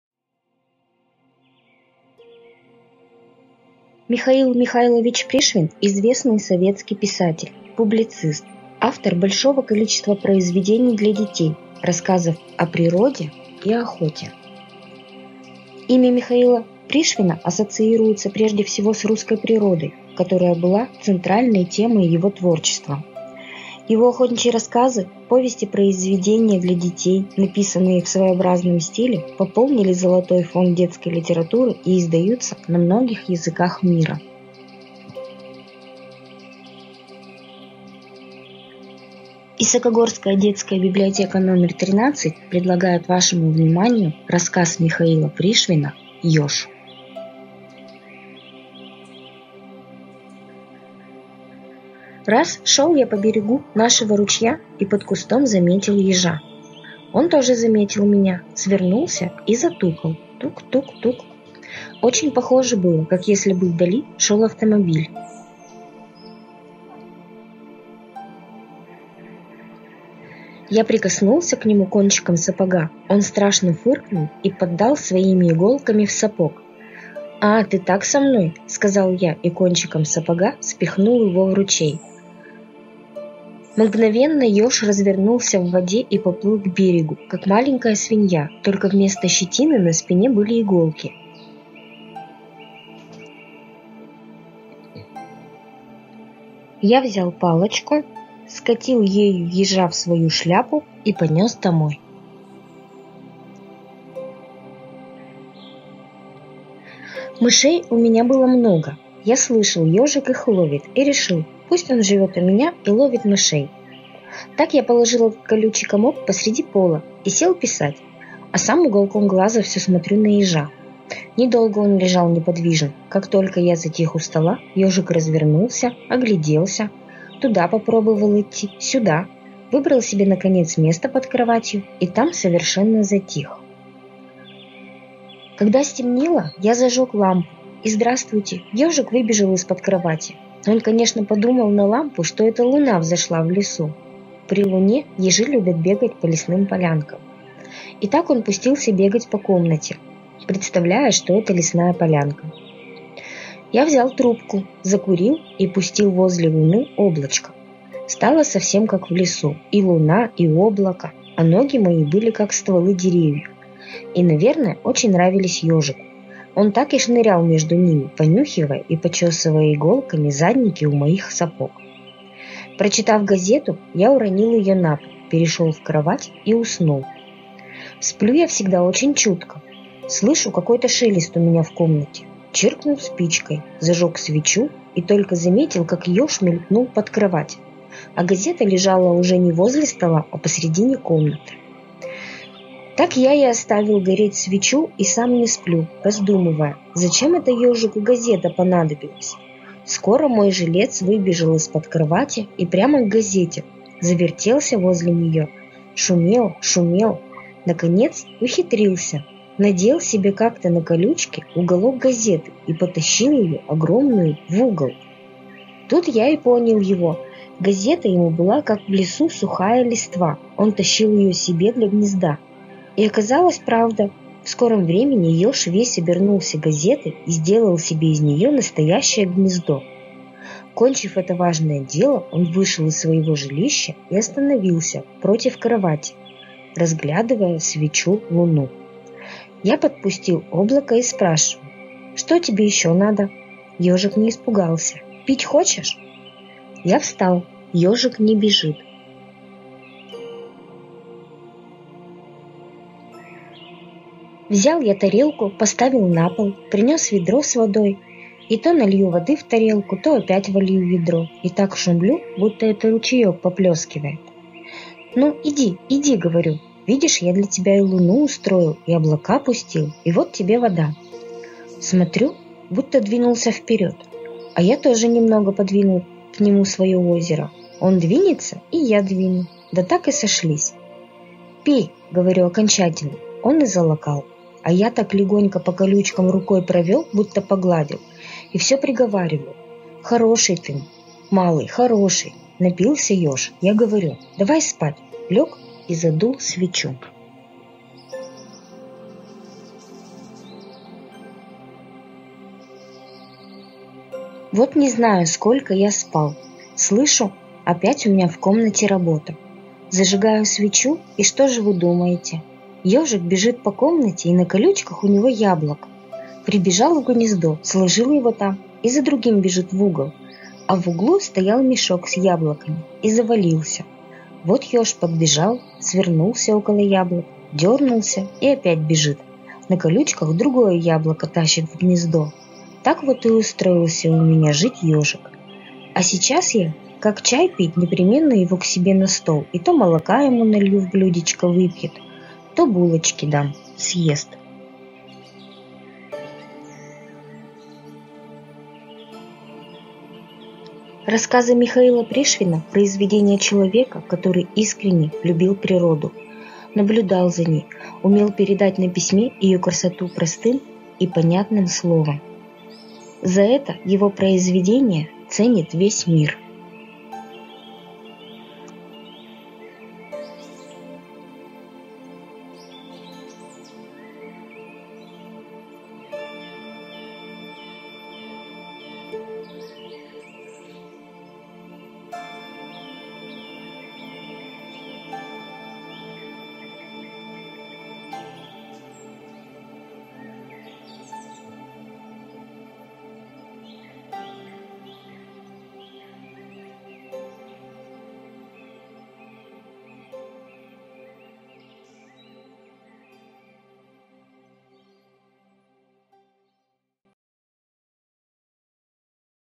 Михаил Пришвин «Ёж»
Сотрудники Исакогорской детской библиотеки № 13 читают рассказ писателя-натуралиста.